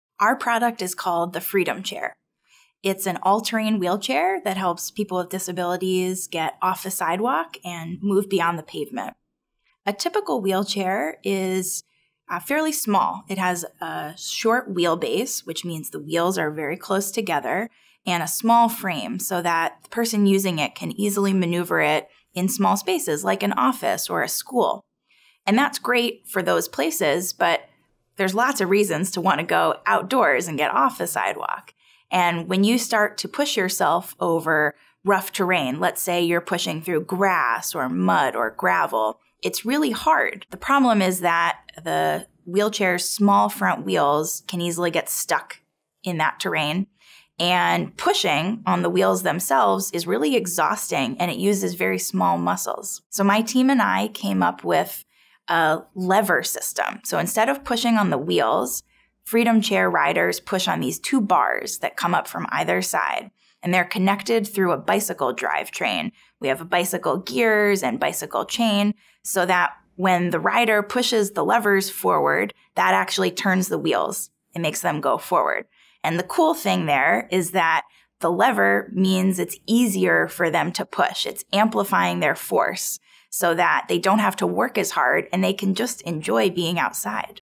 Entrepreneur: What is The Freedom Chair - Tish Scolnik Career Girls Role Model
Find out what it's like to be an Entrepreneur.

Watch Tish Scolnik a Mechanical Engineer and Entrepreneur who started a company called Grit. Her company makes all-terrain, mountain bike-type wheelchairs for people with disabilities. The most rewarding thing about her work is helping people with something that she created.